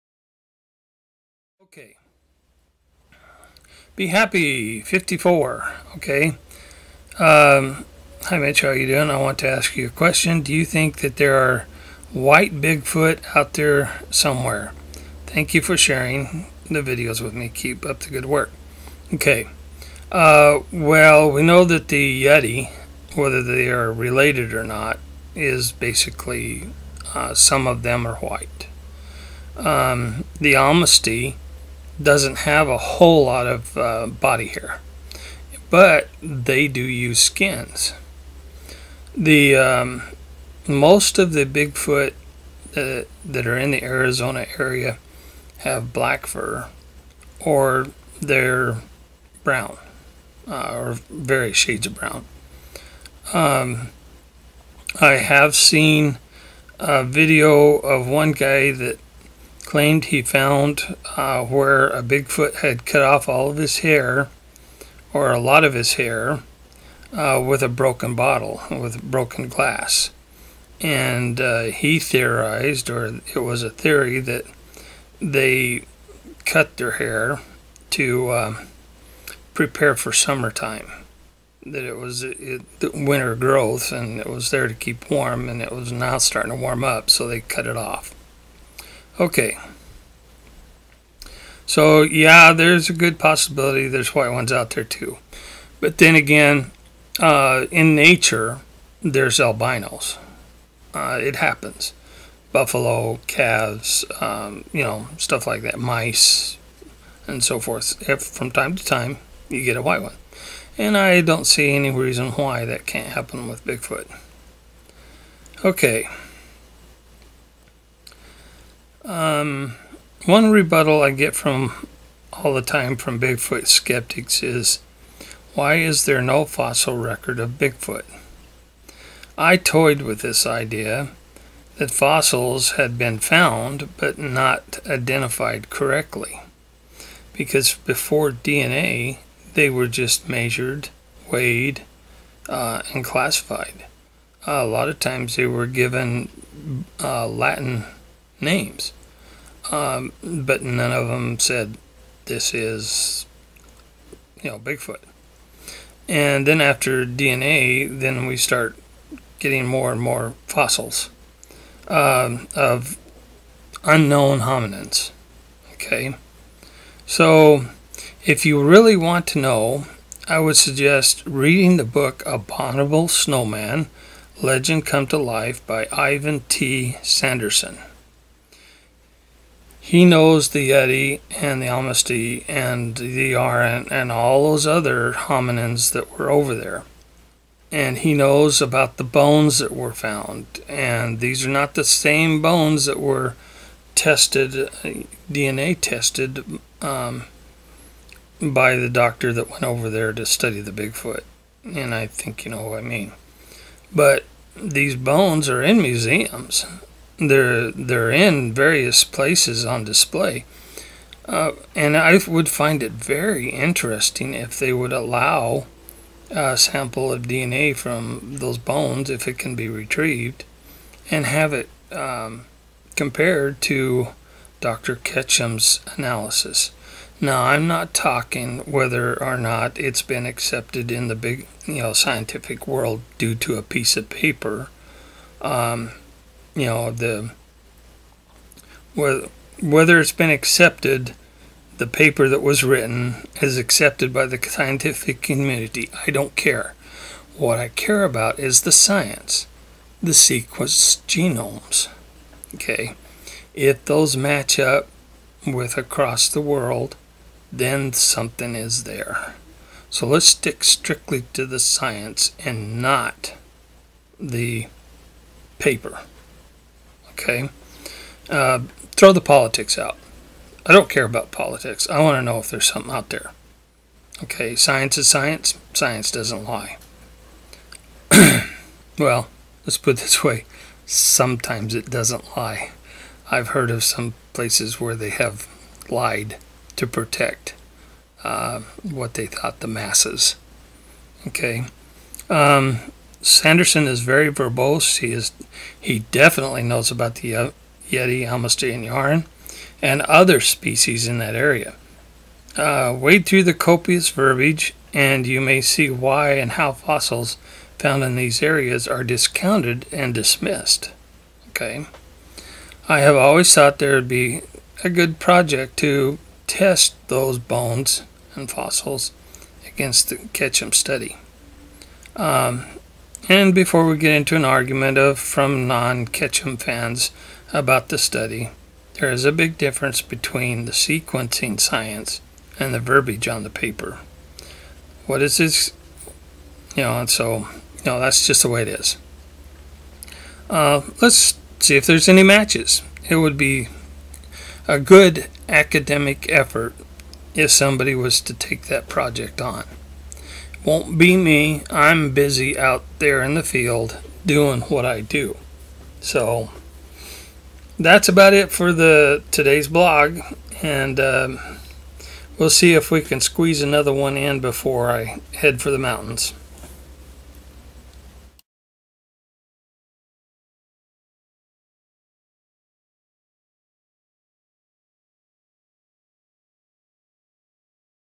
BIGFOOT RESEARCH QUESTION VIDEO BLOG 16 DECEMBER 2014
The Bigfoot Research Question Blog continues.  Questions from you answered by Mitch Waite the best he can.  The answers are based on his opinion which may be changed as more data and information is collected.